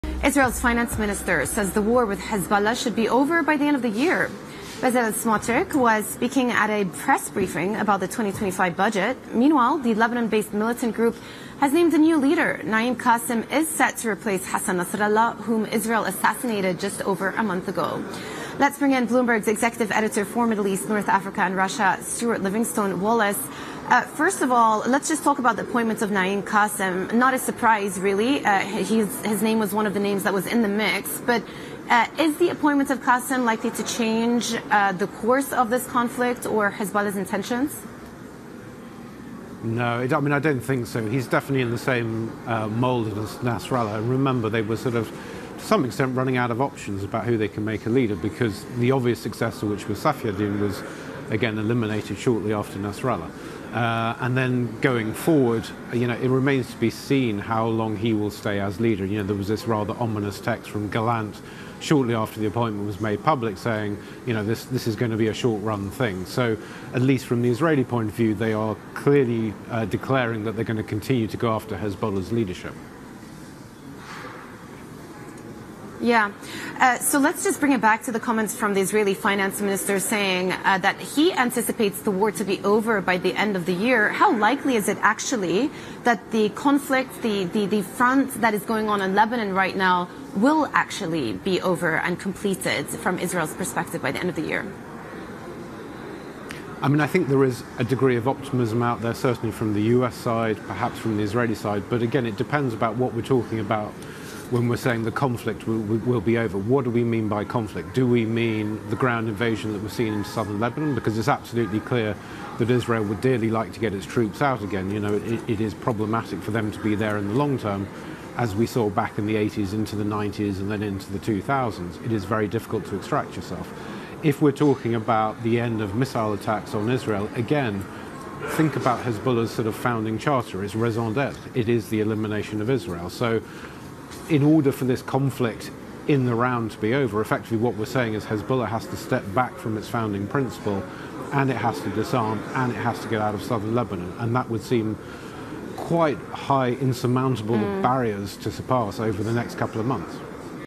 Hezbollah Announces Qasem to Replace Nasrallah as Leader
Hezbollah announced Naim Qasem will become the militant group's new leader, replacing long-standing chief Hassan Nasrallah, who Israel killed just over a month ago. Meanwhile, Israel's Finance Minister Bezalel Smotrich said on Tuesday the war with Hezbollah will likely be over by the end of December. Bloomberg's Stuart Livingstone-Wallace reports.